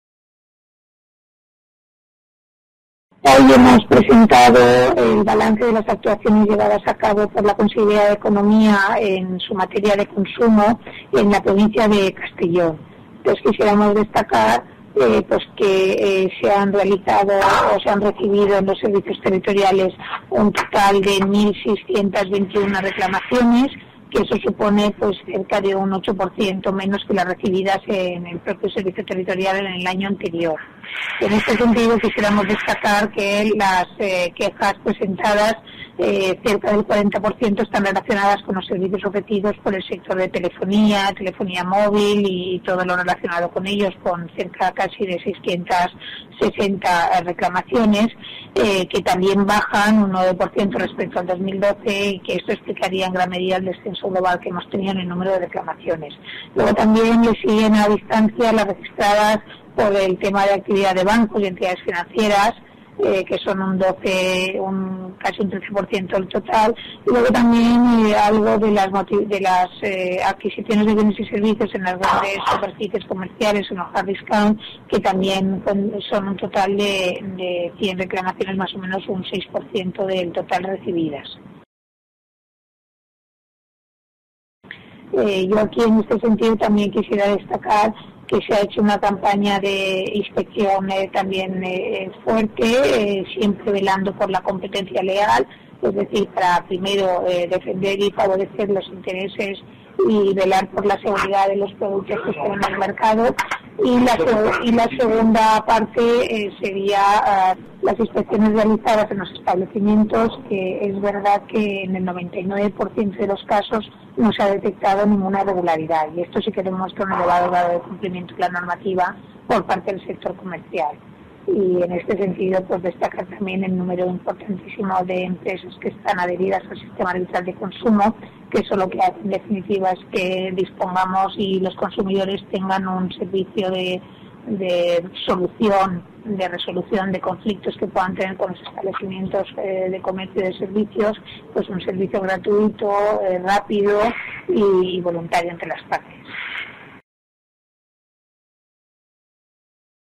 8,1% menos reclamaciones de consumidores y usuarios en Castellón en 2013
La directora general de Comercio y Consumo, Silvia Ordiñaga, ha informado que la Conselleria de Economía, Industria, Turismo y Empleo ha atendido un total de 1.621 reclamaciones presentadas por los consumidores y usuarios en la provincia de Castellón durante 2013, un 8,1% menos que las recibidas por los servicios territoriales en el año anterior.